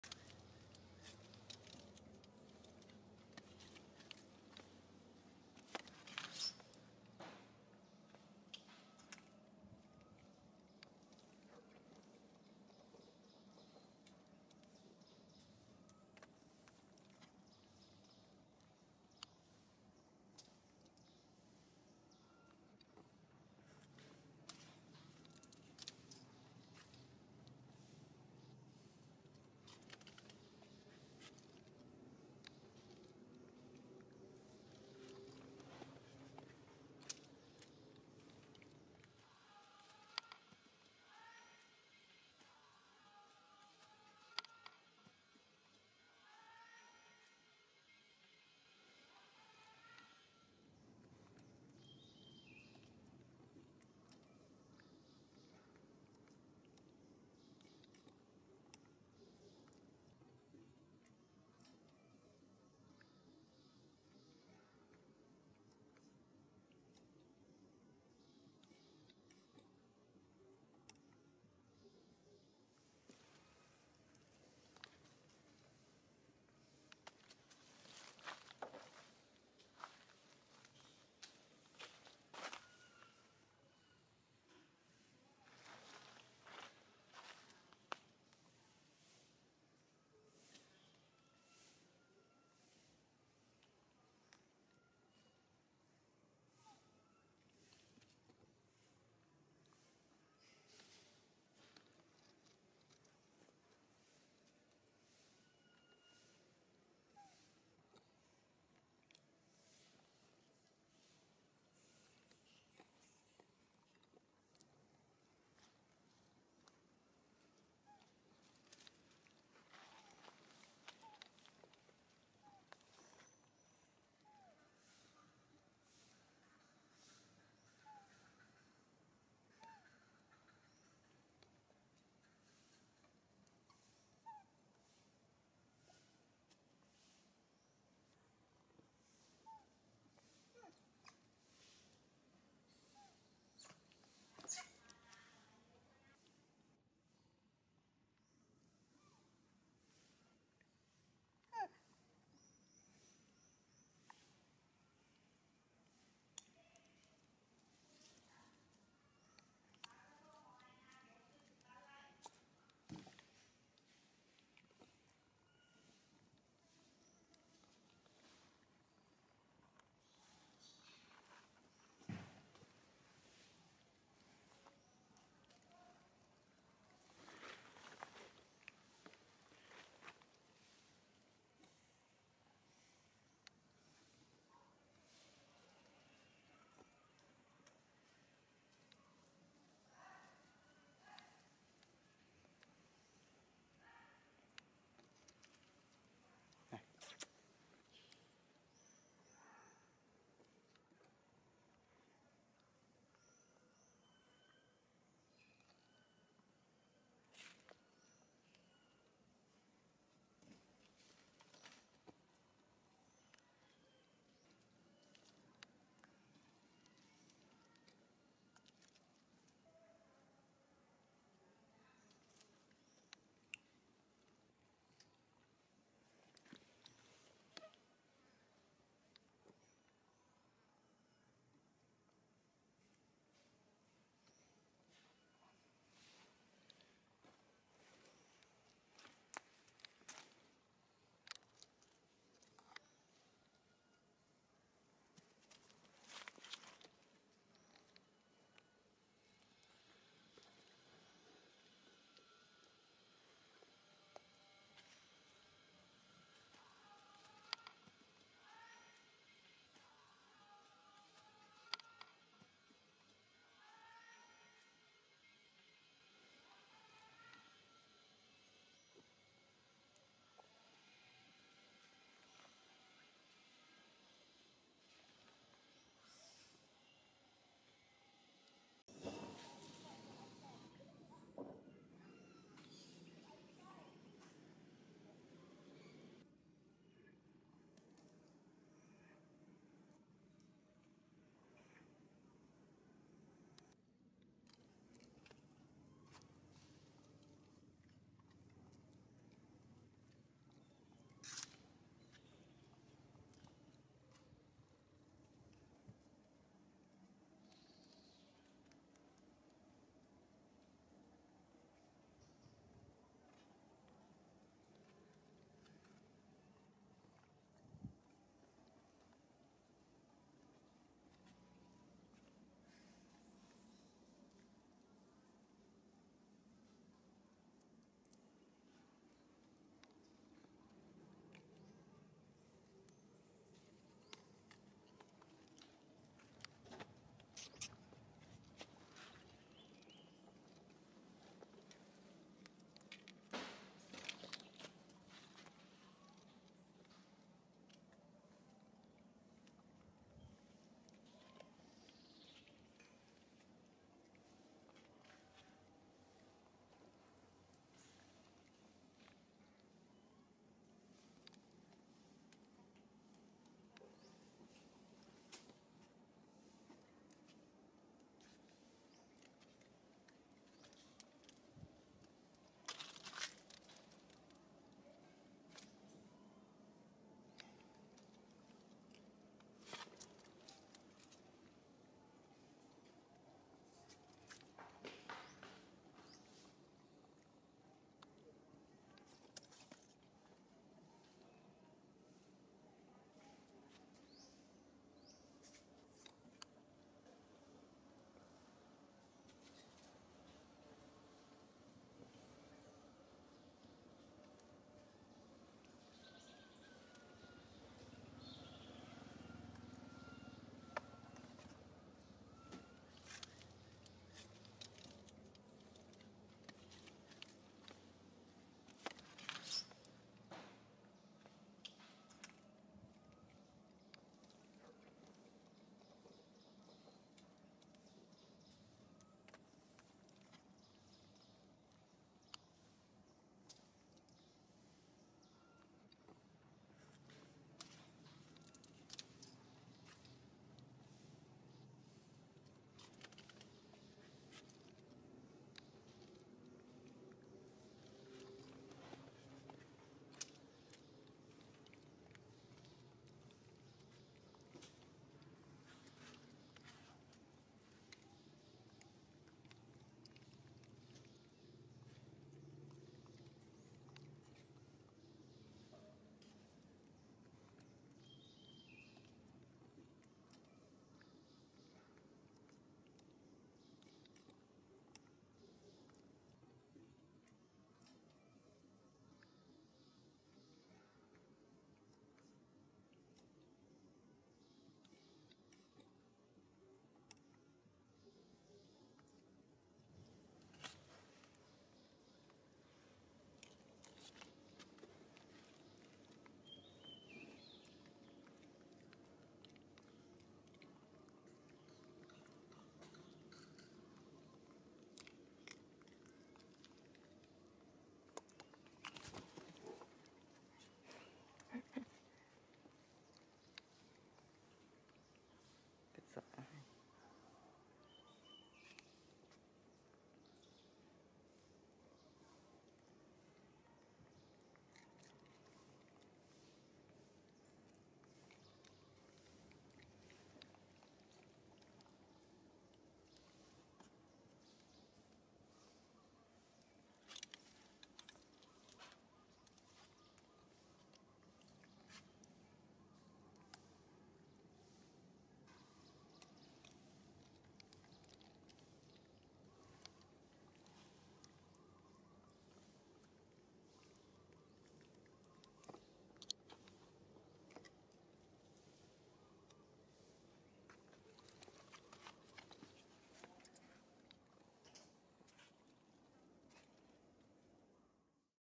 2 in 1...baby monkey SARO play with mom and protect his food from mom SARIKA.
I want to show you about: 2 in 1...baby monkey SARO play with mom and protect his food from mom SARIKA.